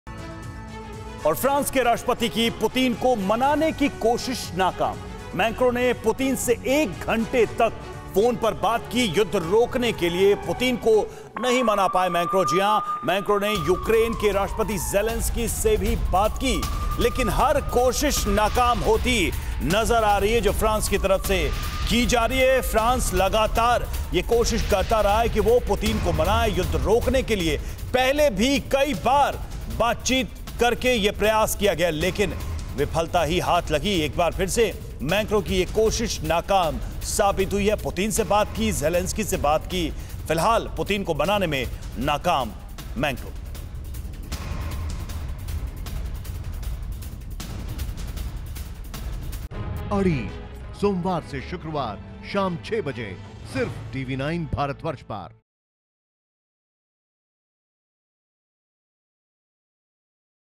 Russia-Ukraine War: Emmanuel Macron की Putin को मनाने की कोशिश नाकाम
इस युद्ध को रोकने के लिए रूस और यूक्रेन के बीच पांच दौर की वार्ता हो चुकी है, लेकिन इस बातचीत से कोई खास समाधान सामने निकलकर नहीं आया है.इस बीच फ्रांस के राष्ट्रपति की पुतिन को मनाने की कोशिश नाकाम रही.मैक्रों ने पुतिन से 1 घंटे तक फोन पर बात की थी.